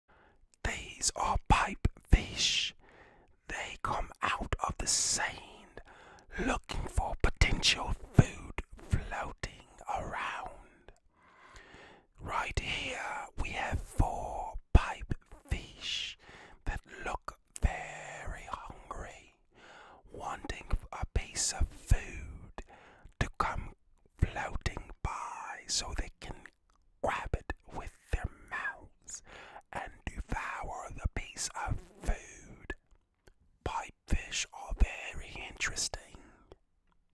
School Of PipeFish Looking All Around To See What Is Going On in (ASMR)
This video is about School Of PipeFish Looking All Around To See What Is Going On in (ASMR) also called autonomous sensory meridian response .